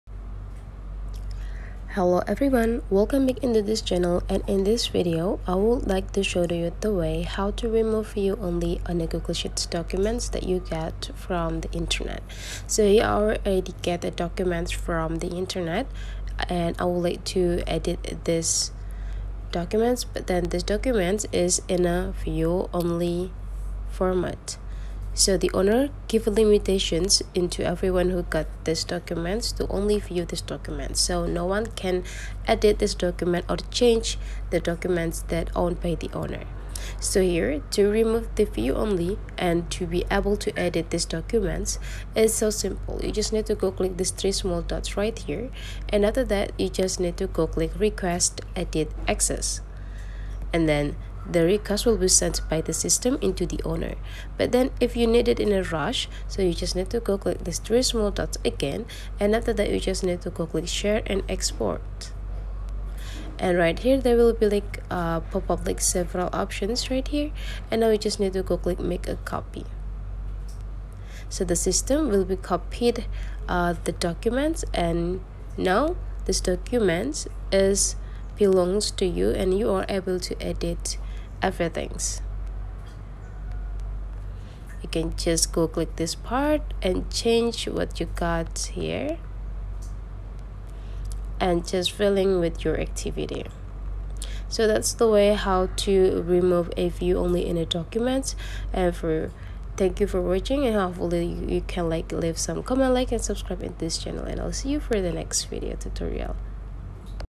❤️ APP HACKS: How To Remove View Only in Google Sheets | Viewer, Editor Permission in Google Sheet
Find a good spreadsheet file but you are not able to edit them is not a problem. Google sheet provides the user to limit the editing access over the documents that they make. They also can give a permission to do collaboration work in one documents. Here is the tutorial to remove view only in Google sheet documents